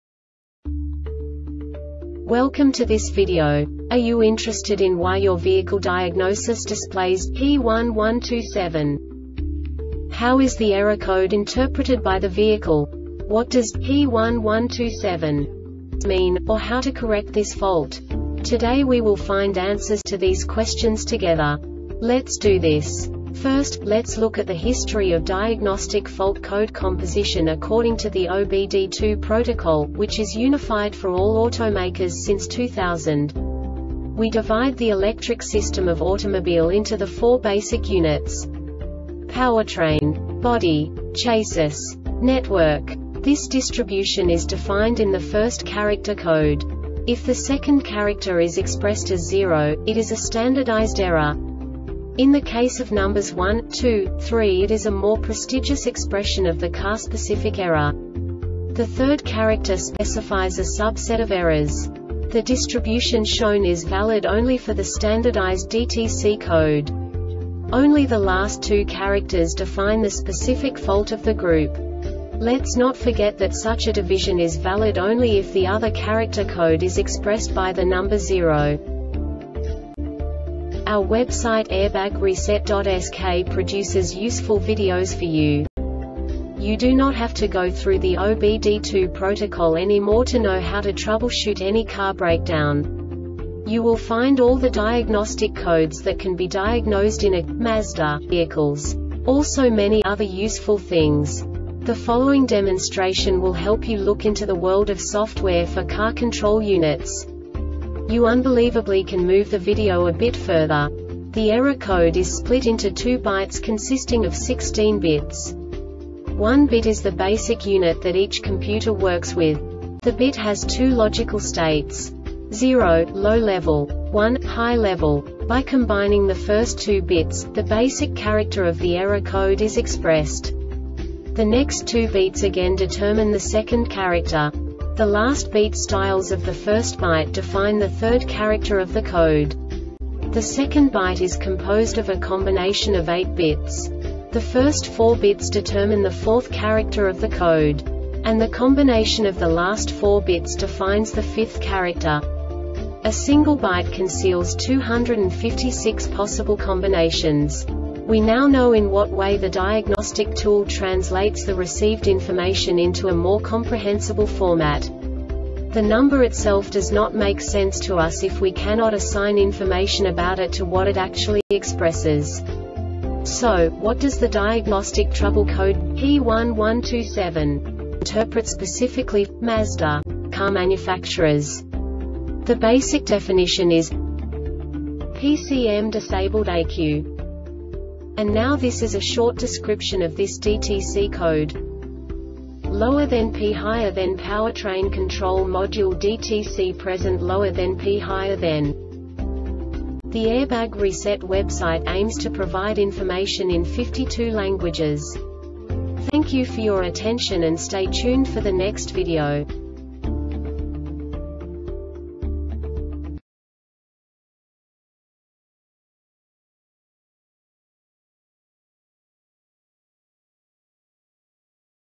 DTC Mazda P1127 Short Explanation
Contents:
0:21 Basic DTC analysis according to OBD2 protocol standard.
1:48 Insight into programming
2:58 A diagnostically understandable description of the Diagnostic Trouble Code
3:05 Basic error definition

Make:
Mazda
Code:
P1127
Definition:
PCM Disabled ECU
Cause:
 lower thenp higher then Powertrain Control Module Dtc Present lower then or p higher then